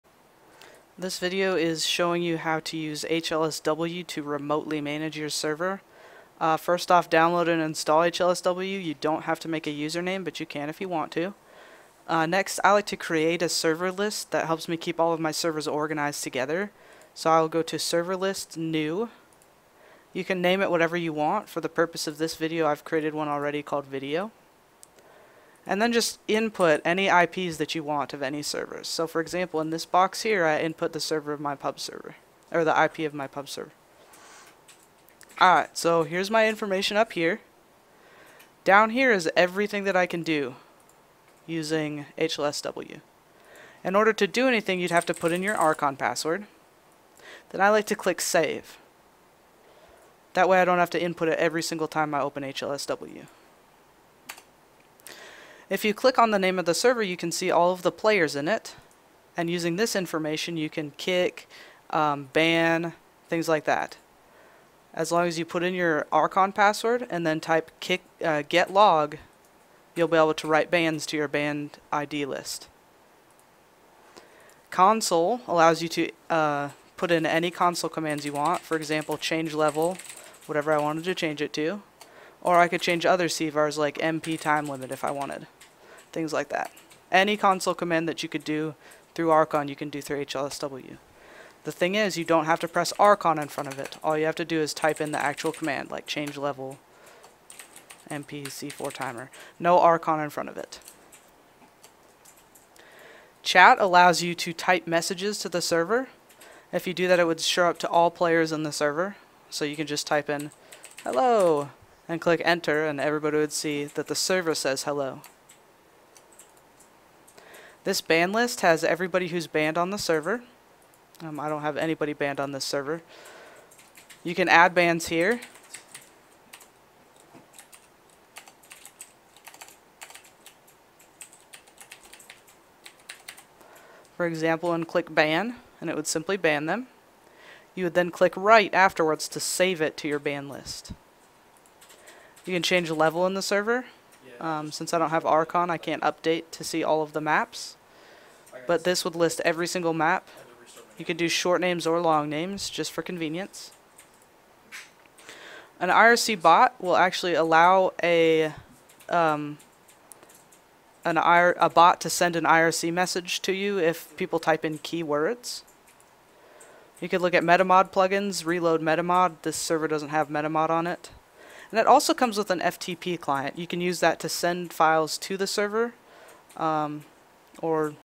Using HLSW for remote server management
A very short and basic tutorial of how to use HLSW to remotely manage your game servers.

absurdminds.net